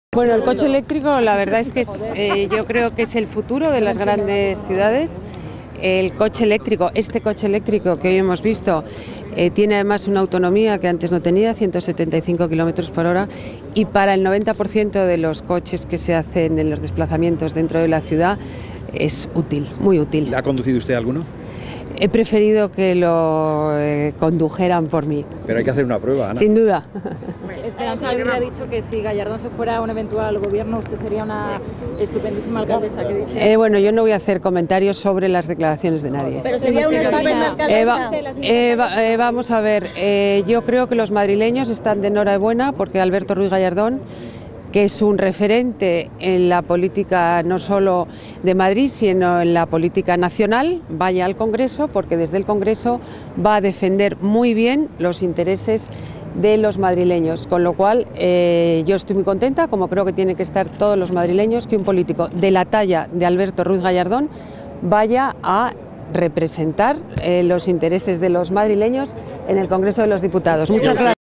Ana Botella no se pronuncia sobre su futuro
La delegada de Medio Ambiente y Movilidad del Ayuntamiento de Madrid, Ana Botella ha optado este lunes por no hacer declaraciones sobre el papel que tendrá en el Consistorio en el caso de que el alcalde de la capital, Alberto Ruiz-Gallardón, deje la Alcaldía con destino a la política nacional.